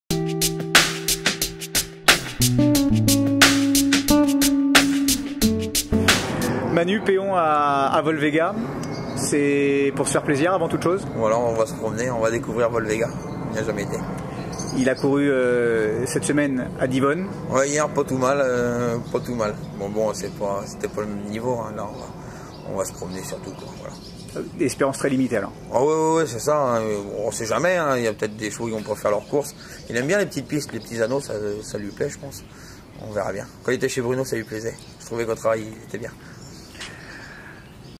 GP VICTORIA PARK 2013 - E. ALLARD (.NL) FOR PEON FOR CLARA
Emmanuel ALLARD présente PEON FOR CLARA dans le Grand Prix Victoria Park 2013.
Ses ambitions seront très limitées...